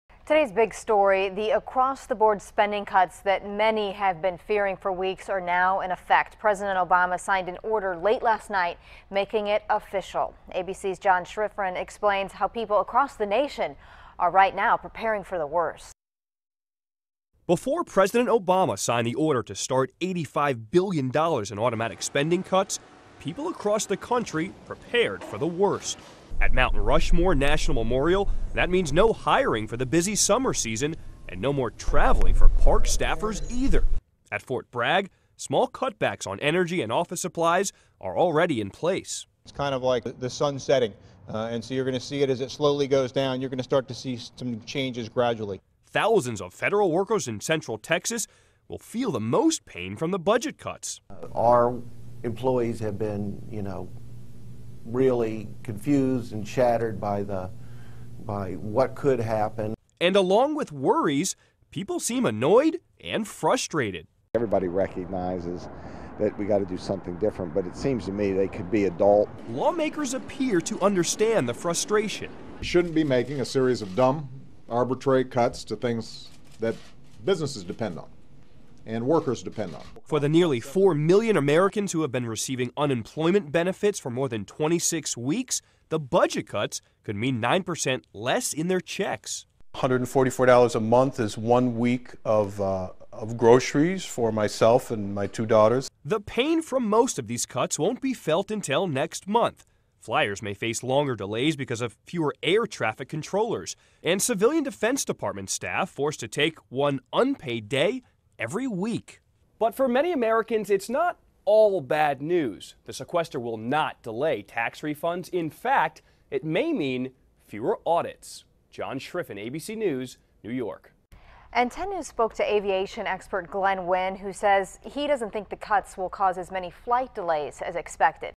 How budget cuts could affect you
Automatic spending cuts that took effect Friday are expected to touch a vast range of government services.